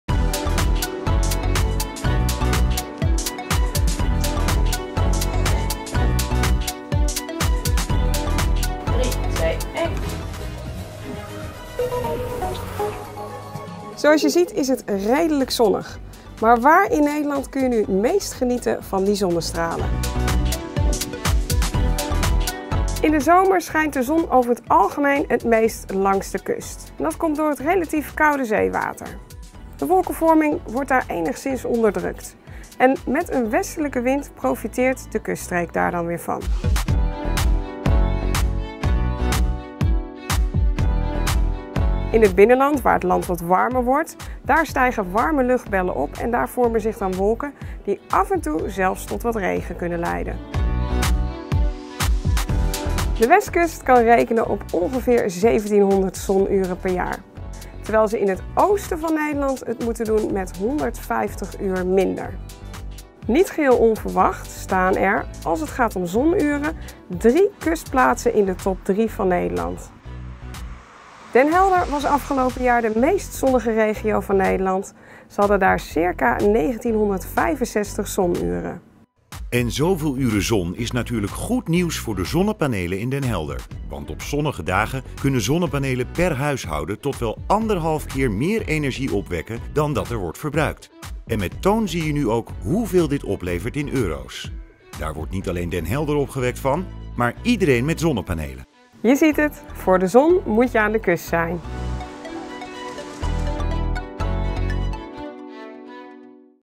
De meest zonnige plekken in Nederland
Het RTL weer onderzoekt samen met Eneco wat de meest zonnige plekken in Nederland zijn en wat de zon oplevert.